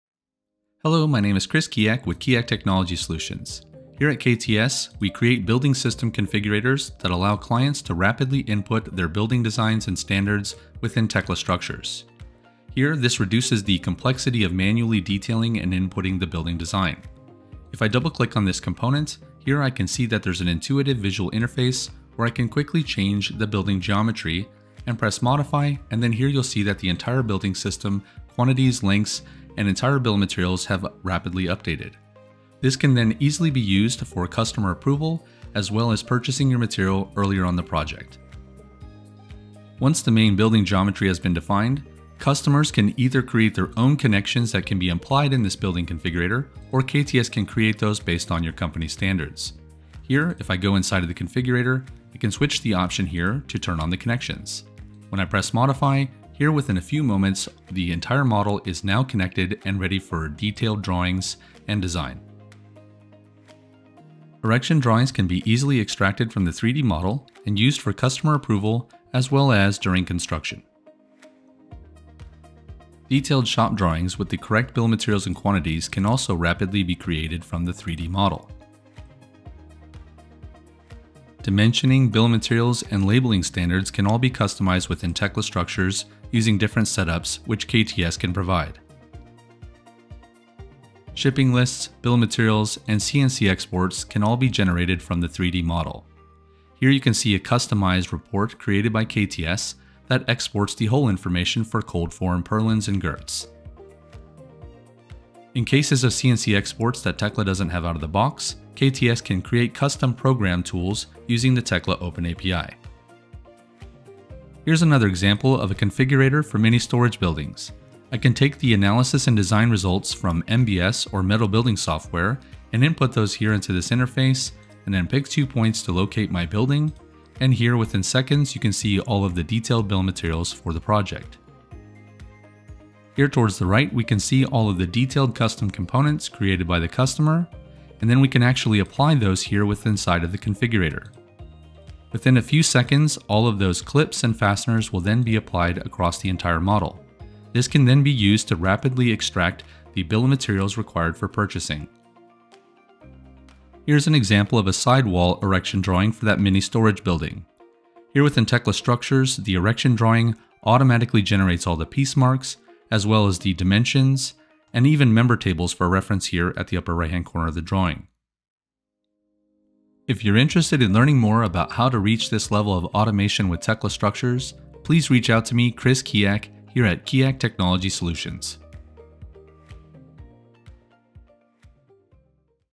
KTS - Building System Configurators in Tekla Structures
This video demonstrates some building system configurators that Keyack Technology Solutions has programmed for customers on top of Tekla Structures using their Application Programming Interface (API). On top of simplifying and expediting building input, detail shop and erection drawings as well as BOM reports and CNC files can be extracted from the model. In this video you'll see examples of a Wide Rlange Gable Roof building configurator and Mini Storage building configurator that have been custom developed for clients.

Chapters
00:00 - Gable Roof Building Configurator
00:38 - Apply Connections
01:03 - Erection Drawings
01:11 - Shop Drawings
01:30 - BOM Reports and CNC Exports
01:53 - Mini Storage Building
02:12 - Apply Connections
02:33 - Sidewall Erection Drawing
02:48 - Conclusion